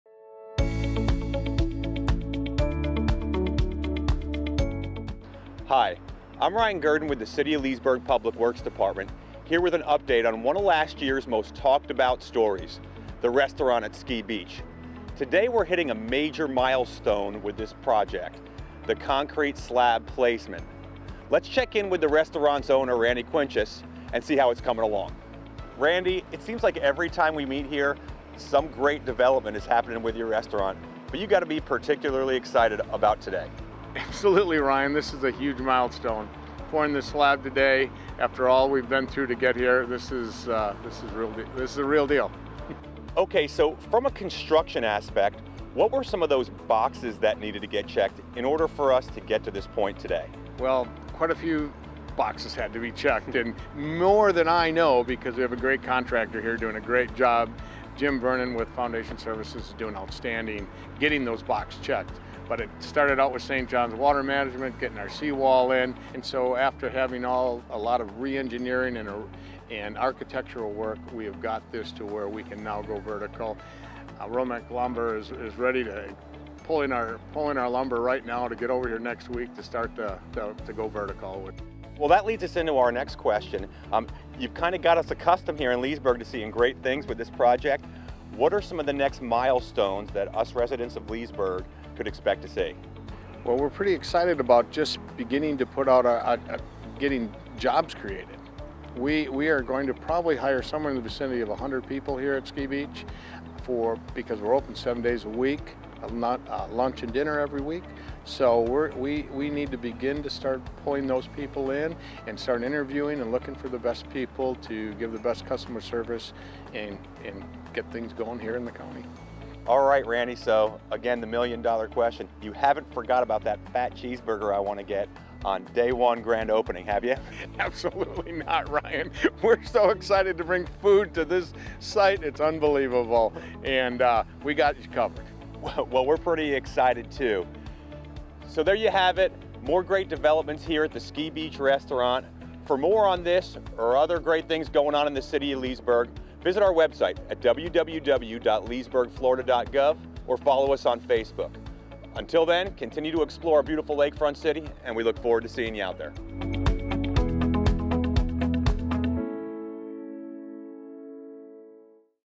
Leesburg Public Works Update Ski Beach Bar Grill
Progress is underway for the Ski Beach Bar and Grill. at Venetian Gardens which is set to open sometime Summer 2022. They hit a major milestone this week. with the pouring of their foundation and will soon begin the building process. Ryan Gerdon has an update.